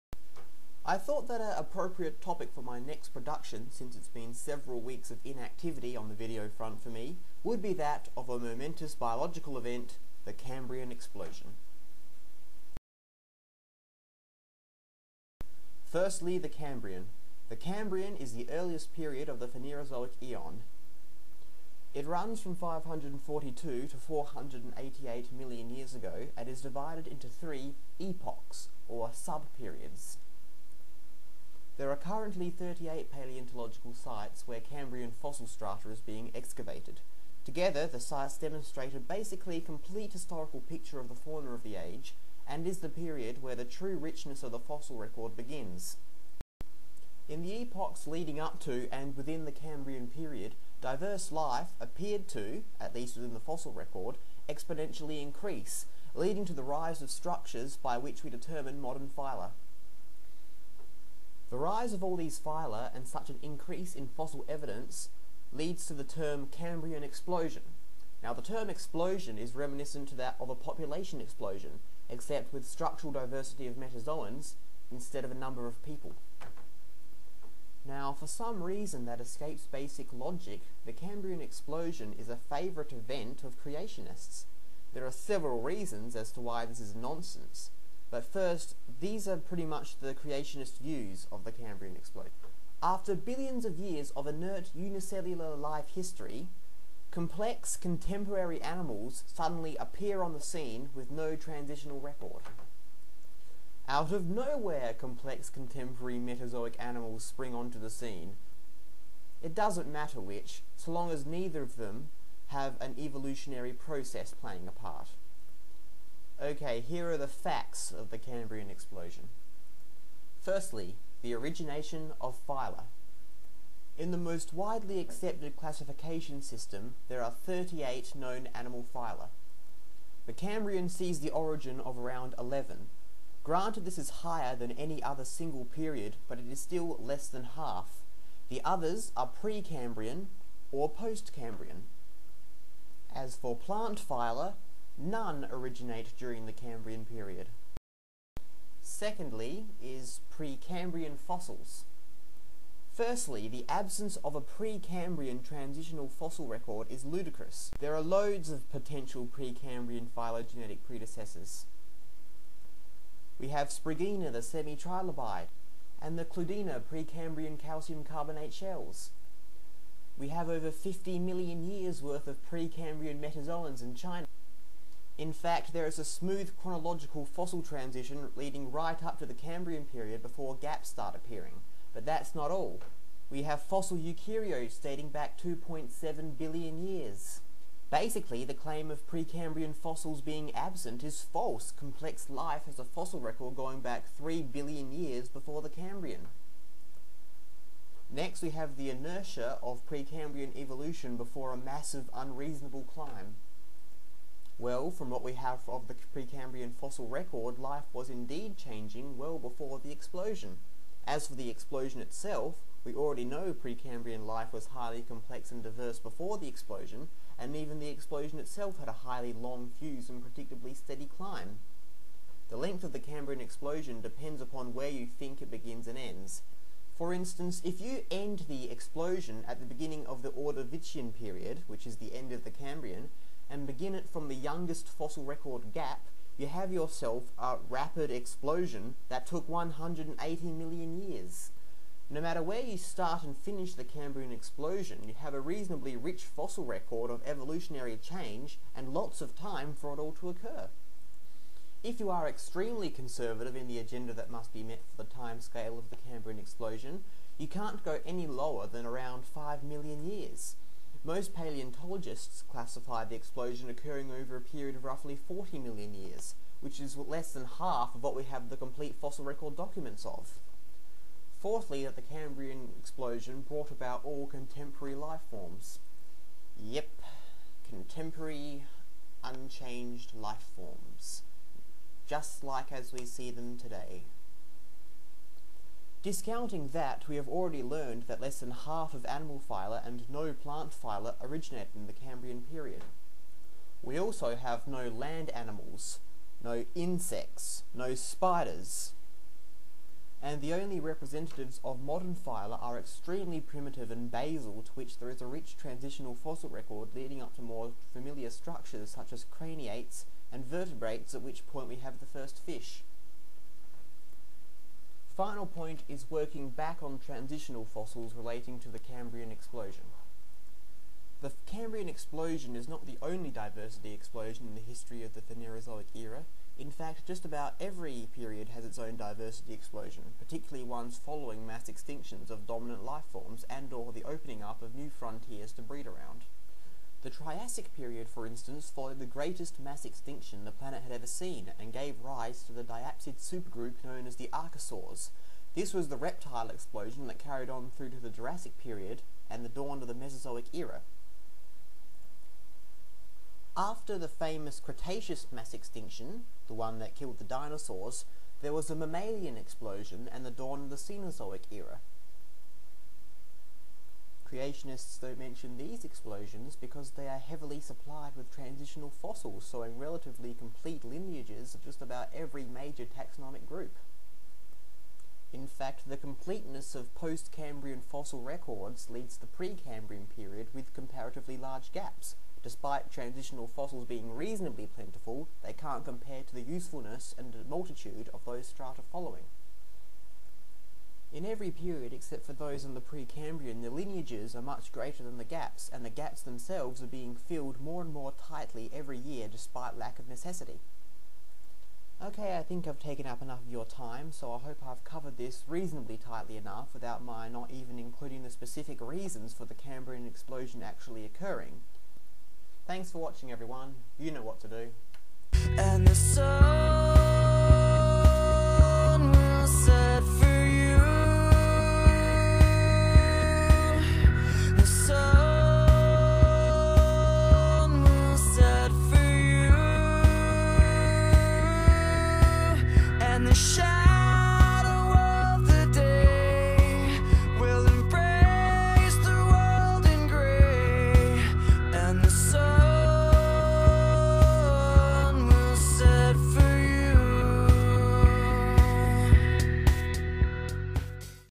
The Cambrian Explosion
A video on the Cambrian Explosion

For some more information an fossils I havefound this article after releasing the video

Note that is uses a different phyla classification system and says the Cambrian period gave rise to around nine phyla rather than the 11 from my source.

Because of the gradient of classification this kind of arbitrary difference is expected.

An excellent rebuttal to the Cambiran explosion was constructed by Kevin Padian for the Dover Trial

transcript and slides feature here